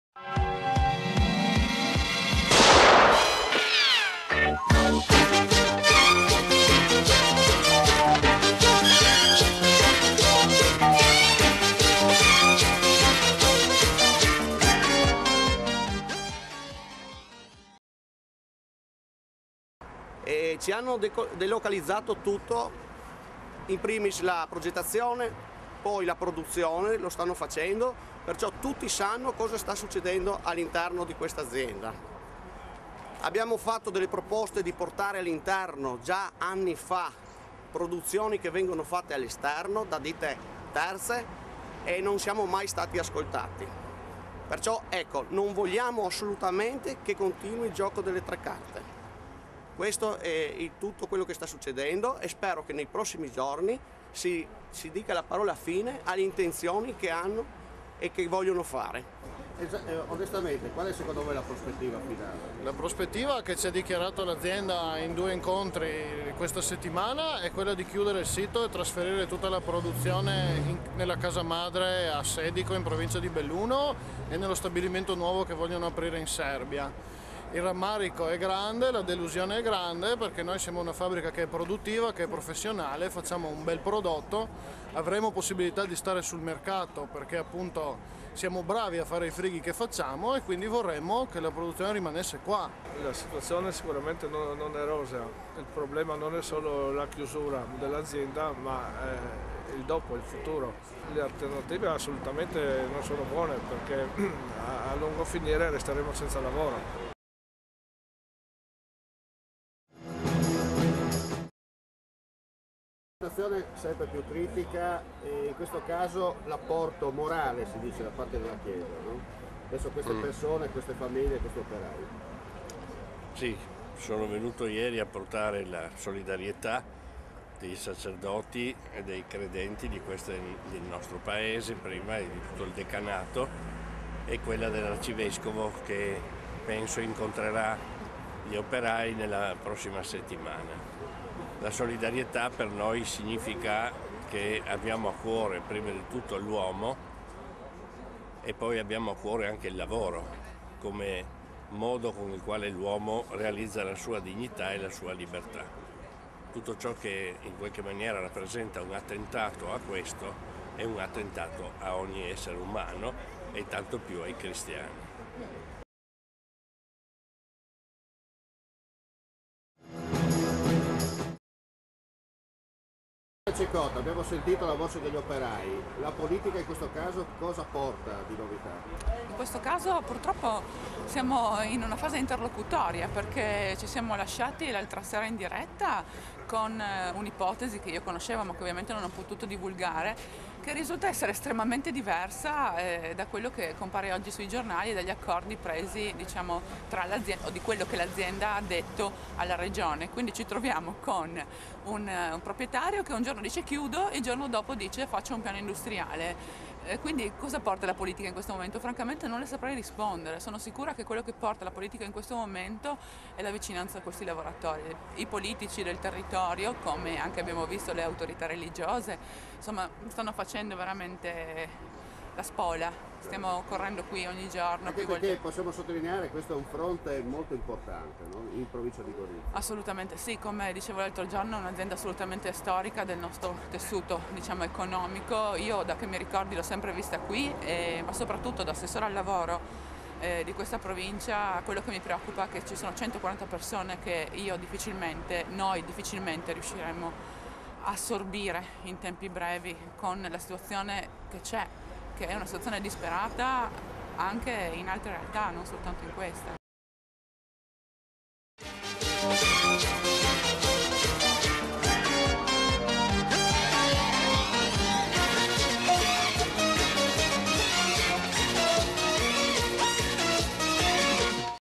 15 02 2014 Speciale Crisi Lavoro DE RIGO Ronchi del Legionari GO
Vertenza in corso ma soprattutto quale futuro per i 144 operai della DE RIGO di Ronchi dei Legionari? Il servizio